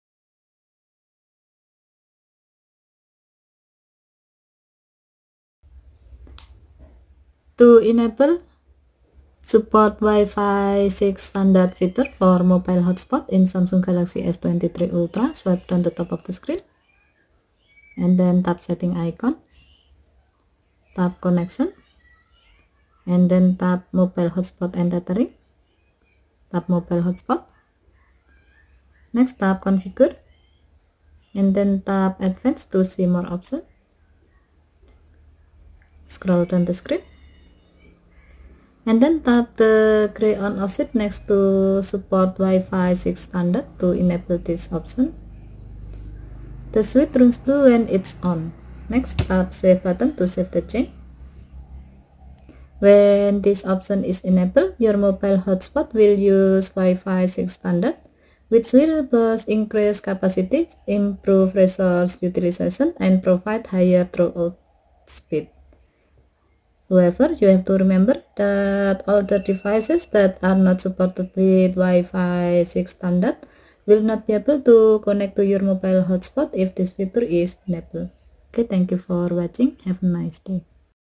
Samsung Galaxy S23 Ultra : How to enable support Wi Fi 6 standard feature for mobile hotspot
In this video, you will find the steps on how to enable support Wi-Fi 6 standard feature for mobile hotspot in Samsung Galaxy S23/S23+/S23 Ultra. When you enable this option, your mobile hotspot will use Wi-Fi 6 standard which will boast increased capacities, improve resource utilization and provide higher throughput speeds. However, you have to remember that older devices that are not supported with Wi-Fi 6 standard will not be able to connect to your mobile hotspot if this feature is enabled This video uses Samsung Galaxy S22 Ultra SM-S918B/DS International version runs with Android 13 (Tiramisu)